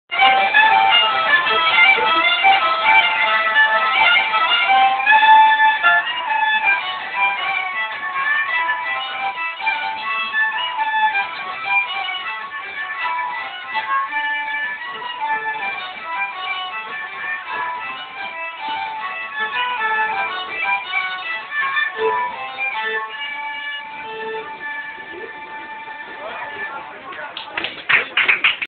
Musicalies 2009 3
Musique au festival des Musicalies en Sologne 2009. Enregistré sur téléphone portable, désolé pour la qualité!